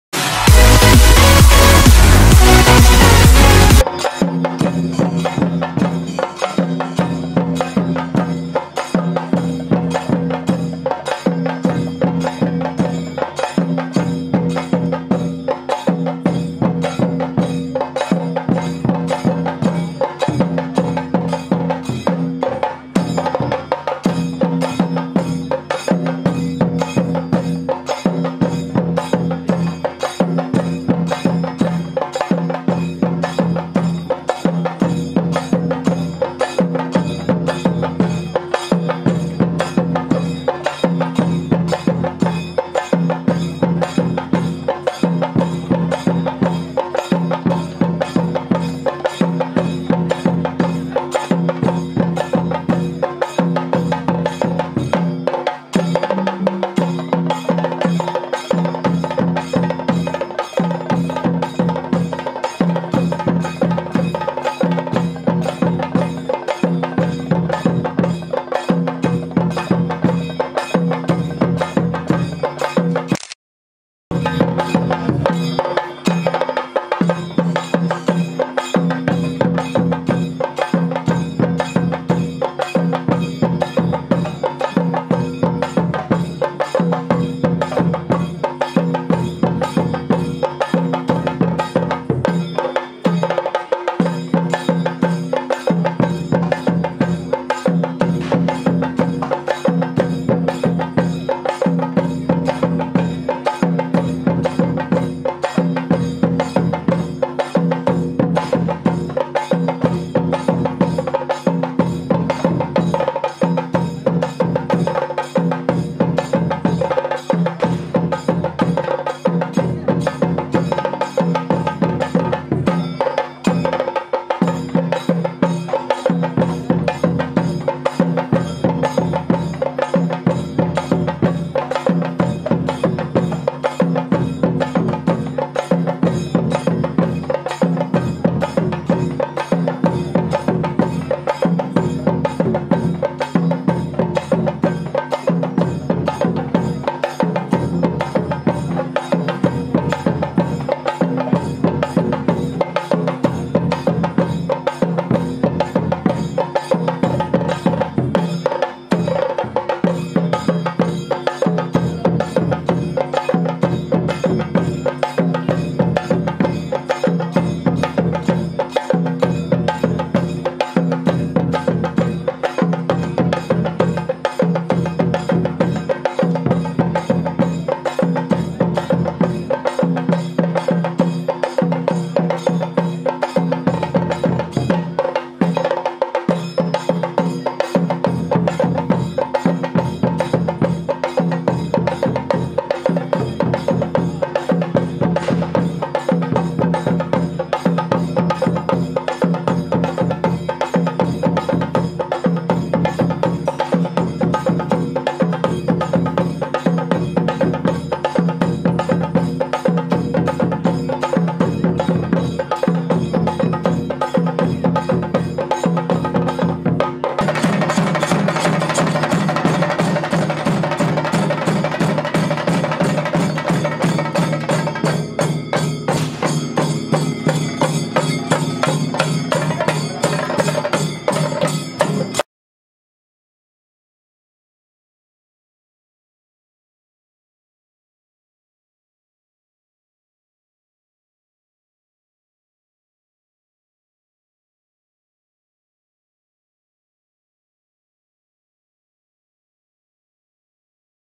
Mayilattam Folk Dance | மயிலாட்டம் | World Popular Mayilattam | Peacock Dance of Kerala performed
Mayilattam is an artistic and religious form of dance performed in the Hindu temples of Tamil Nadu and Kerala in reverence to Lord Subrahmanya.

Mayilattam performers wear costumes from head to toe like peacock with beak, that can be opened and closed using a thread, and perform specific dances. The performers dance on a tall piece of wood attached at the end of their feet. This art requires extensive training and practise. This dance is performed in all Murugan (Lord Subrahmanya) temples as a tradition during festivals. But due to the difficulties in practise and less amount of wages for the dancers, the number of Mayilattam performers is decreasing.